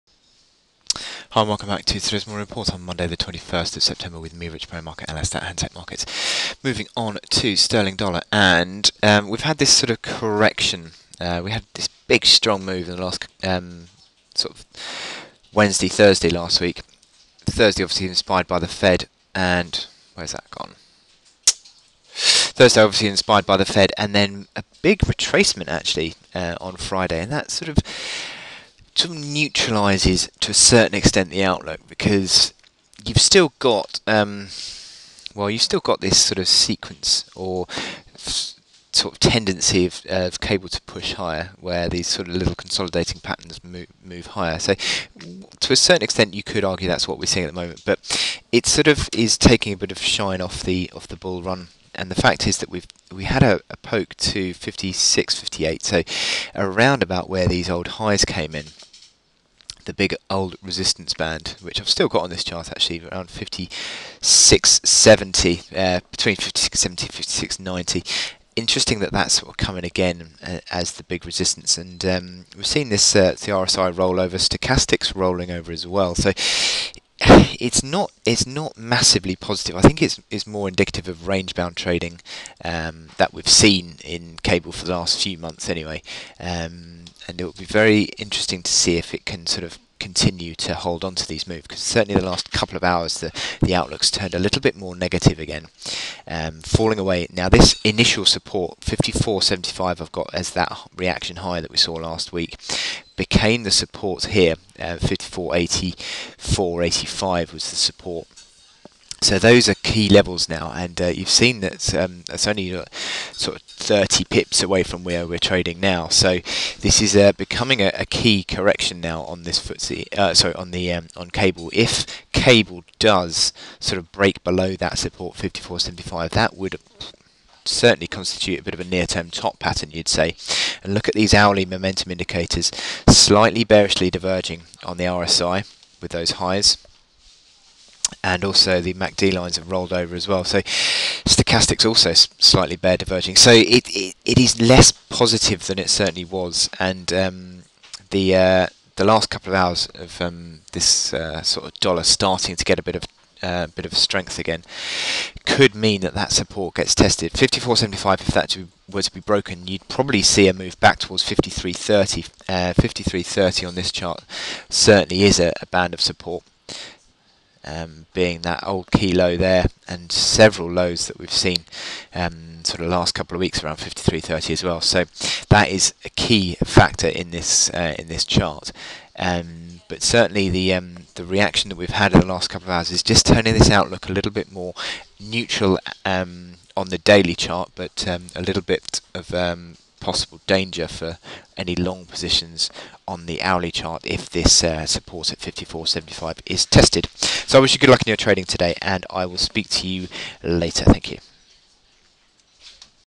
GBP/USD 21/09/2015
Could the bullish outlook be about to come under pressure again as the dollar bulls threaten? What is the support to watch for and what could be the impact on the outlook?